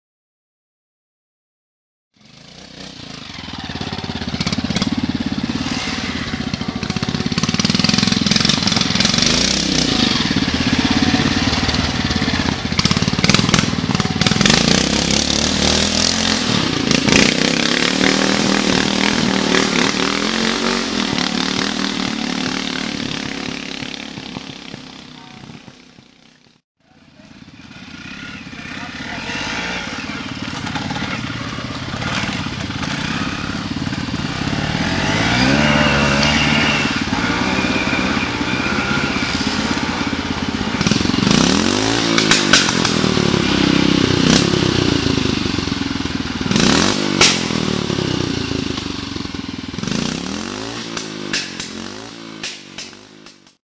Une Triumph ( Benoit Vérin ) et Une SWM rouge et blanc, dans le Pipe Line Arbecey 2016
Le site de vidéos régionales, nationales et internationales